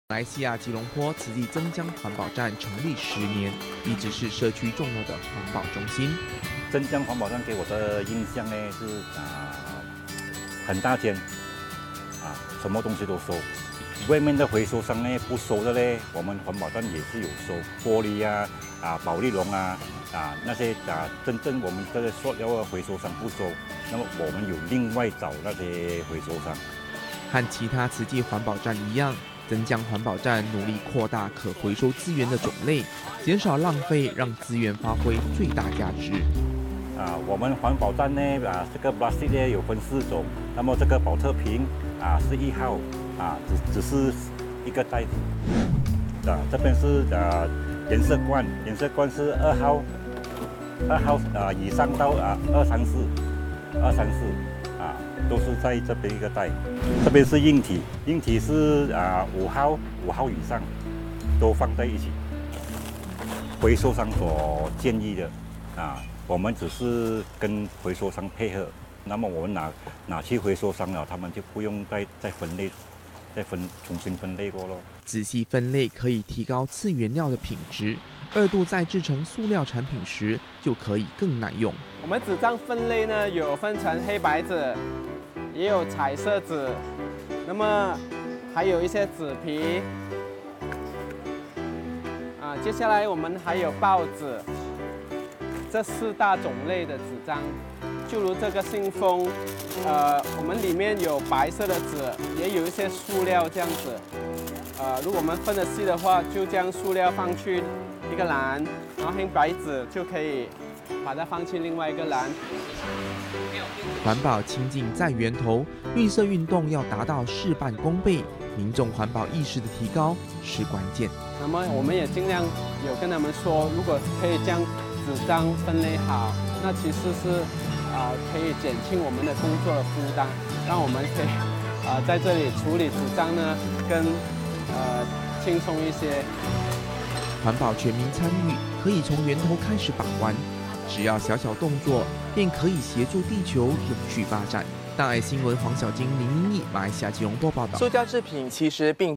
環保站十寒暑 帶動社區環保風
馬來西亞吉隆坡增江環保站，成立十年來，能回收的，絕不拒絕，即使回收商考量成本，志工們思考的是對大地的汙染，分類仔細，做到清淨在源頭，是社區重要的環保中心，也成為大家的典範。

馬來西亞吉隆坡慈濟增江環保站成立10年，一直是社區重要的環保中心。

慈濟志工 黃新順：「 增江環保站給我的印象是，很大間 什麼東西都收，外面的回收商不收的，我們環保站也是有收 玻璃 保麗龍，那些我們塑料回收商不收，那麼我們有另外找那些回收商。」

和其他慈濟環保站一樣，增江環保站努力擴大可回收資源種類，減少浪費，讓資源發揮最大價值。

慈濟志工 黃新順：「我們環保站這個塑膠有分四種，那麼這個寶特瓶是一號，只是一個袋子，這邊是顏色罐 顏色罐是二號，二號以上 二 三 四號，都是在這邊的一個袋，這邊是硬體 硬體是五號，五號以上 都放在一起，(這些都是)回收商所建議的，我們只是跟回收商配合，那麼我們送去回收商，他們就不用再重新分類過。」

仔細分類可以提高次原料品質，二度再製成塑料產品時，就可以更耐用。

慈濟志工 王康健：「 我們紙張分類 有分成黑白紙，也有彩色紙 那麼還有一些紙皮，接下來 我們還有報紙，這四大種類的紙張，就如這個信封，我們裡面有白色的紙，也有一些塑料，如果我們分得細的話，就將塑料放去一個籃，然後黑白紙就可以，把它放去另外一個籃。」

環保清淨在源頭，綠色運動要達到事半功倍，民眾環保意識的提高是關鍵。

慈濟志工 王康健：「 那麼我們也盡量有跟他們說，如果可以將紙張分類好，那其實是可以減輕我們工作負擔，讓我們可以在這裡處理紙張，更輕鬆一些。」

環保全民參與，可以從源頭開始把關。只要小小動作便可以協助地球永續發展。

黃曉菁 林英意 馬來西亞報導